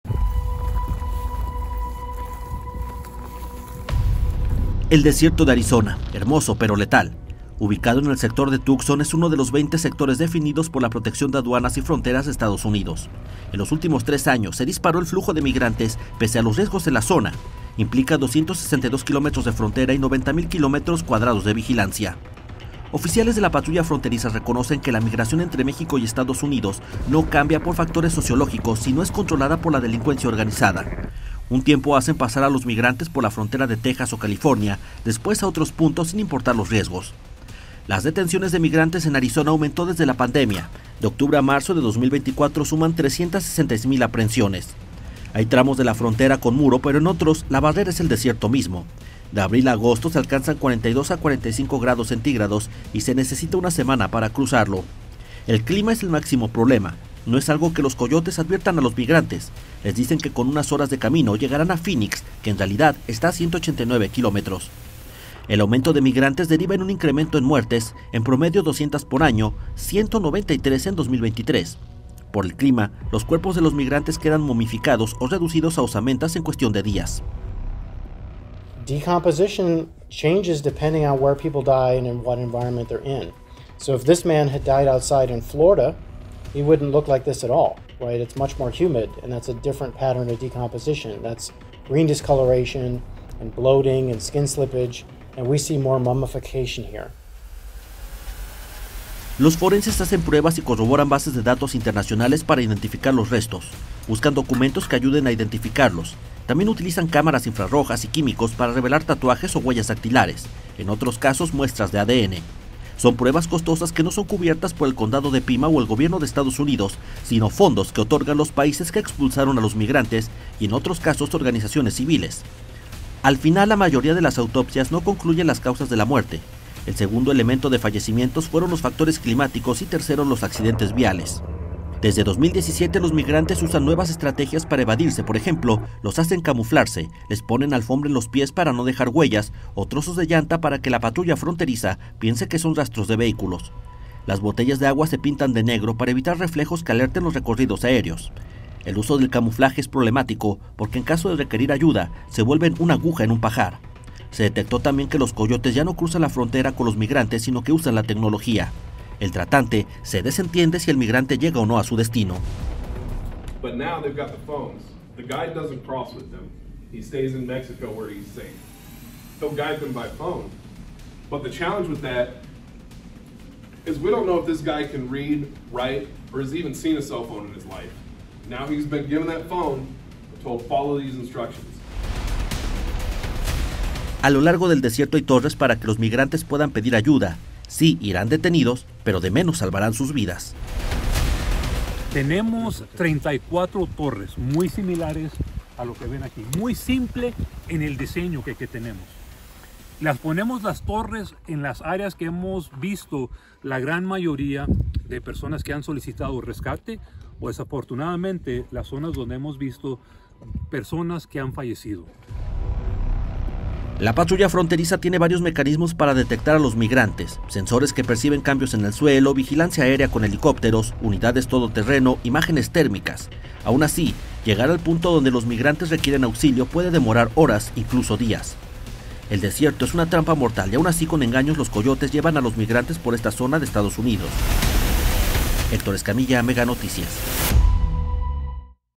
PARTE 1 EL DESIERTO
El desierto de Arizona, hermoso pero letal. Ubicado en el sector de Tucson, es uno de los 20 sectores definidos por la Protección de Aduanas y Fronteras de Estados Unidos. En los últimos tres años se disparó el flujo de migrantes pese a los riesgos de la zona. (Animación 1) Implica 262 kilómetros de frontera y 90 mil kilómetros cuadrados de vigilancia.

Oficiales de la Patrulla Fronteriza reconocen que la migración entre México y Estados Unidos no cambia por factores sociológicos, sino es controlada por la delincuencia organizada. Un tiempo hacen pasar a los migrantes por la frontera de Texas o California; después a otros puntos sin importar los riesgos.